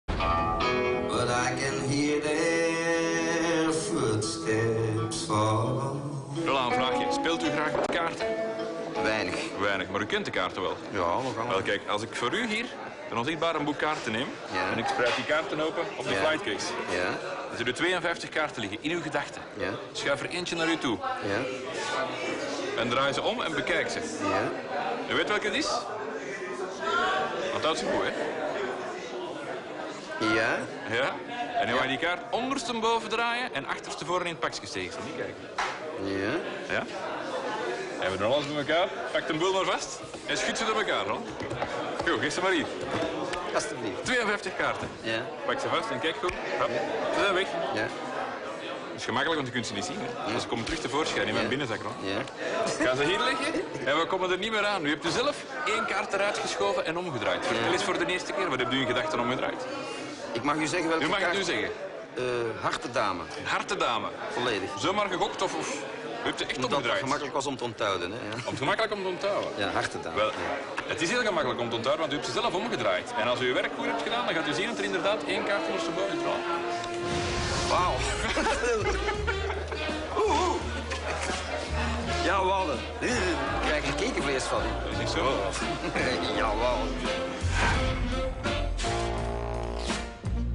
Roland krijgt er 'kiekevlees' van :-)
Leuk experimentje met TOP-muzikant Roland Van Campenhout